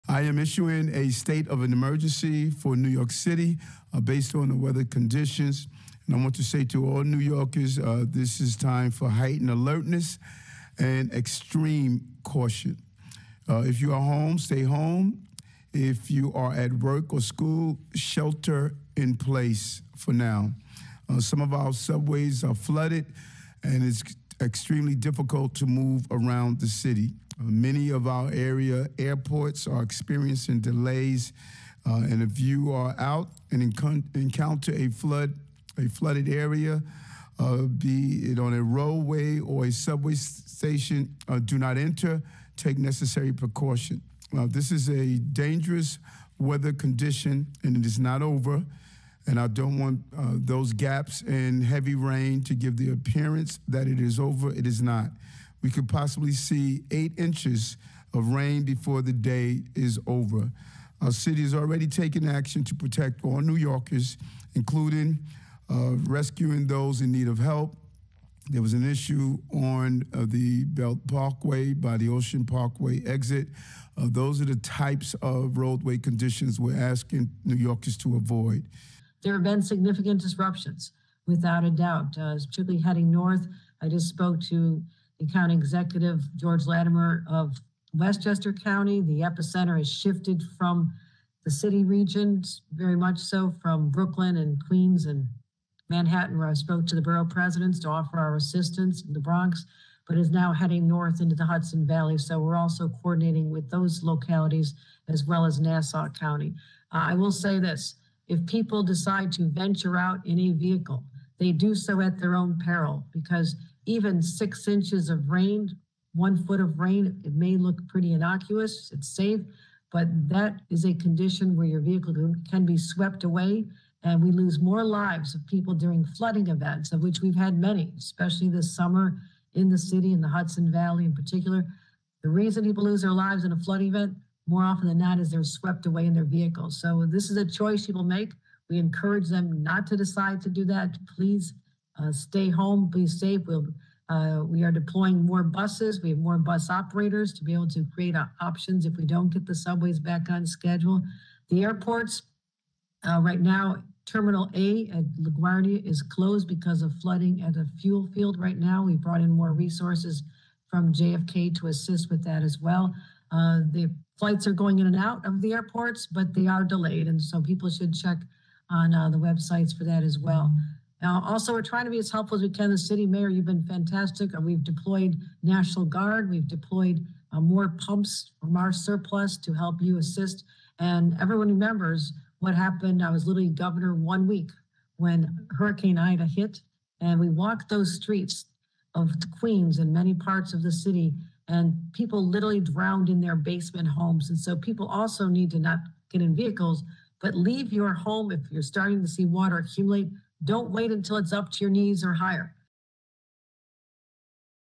NYC Floods: Mayor Adams Declares State of Emergency
New York City Mayor Eric Adams declares a state of emergency on Septemner 29th as heavy rains have caused major flooding throughout the municipality.
Governor Kathy Hochul also discusses the potential dangers north of the city.